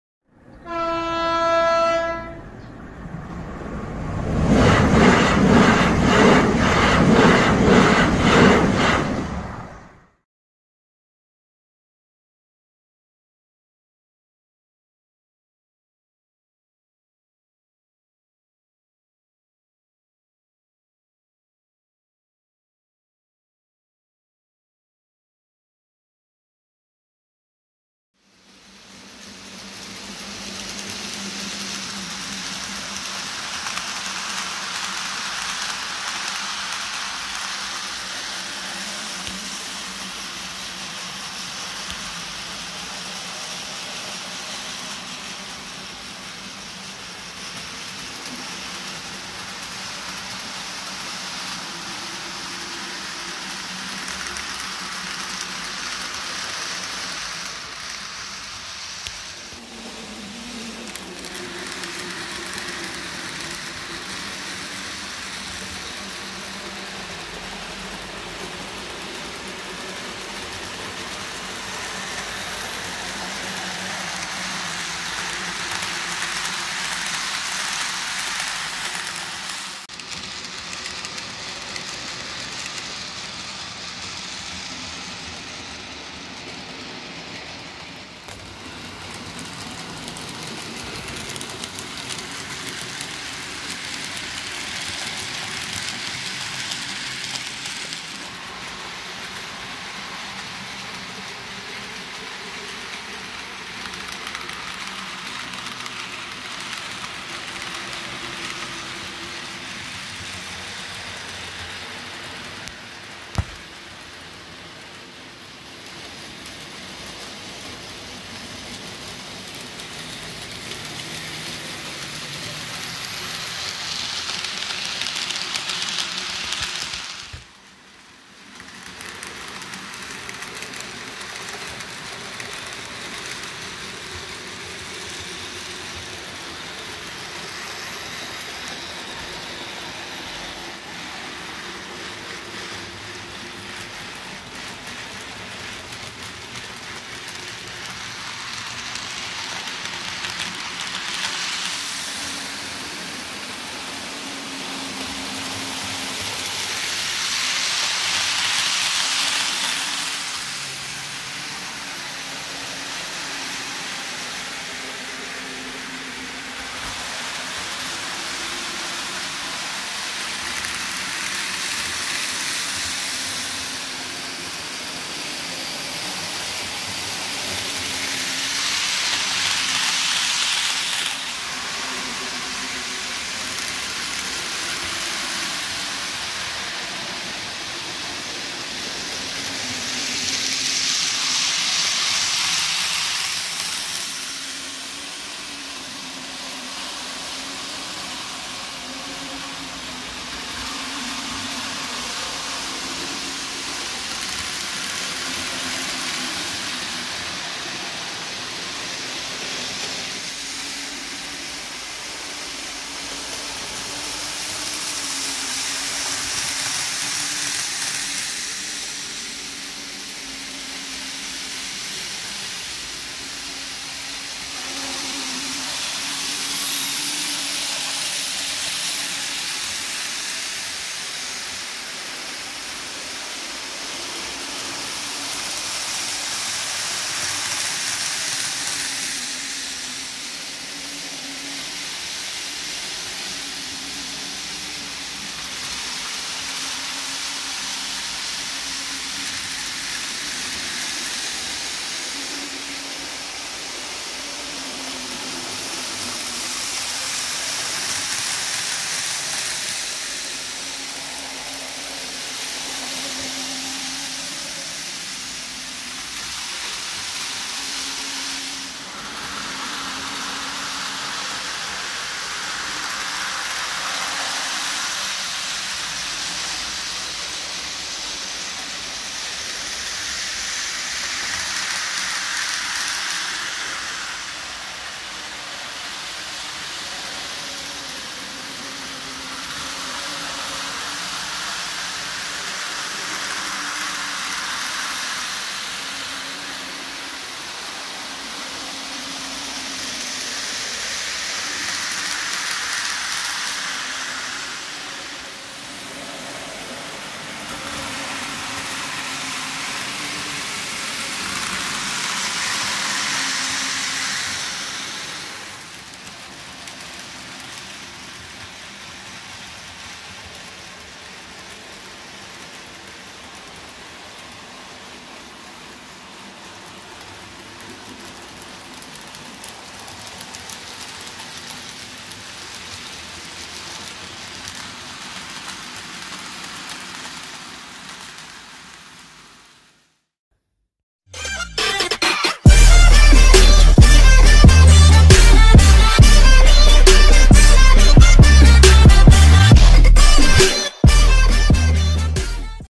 Ep09 - Running Session - Feb 2021 at Ħal-Zuzzu Model Railway
Got my Dapol Class 33 working again.
Decided to take the trains for a spin.
Nothing scale about what you are about to see.
I wanted to test the layout once again.
Some of the footage is at full throttle.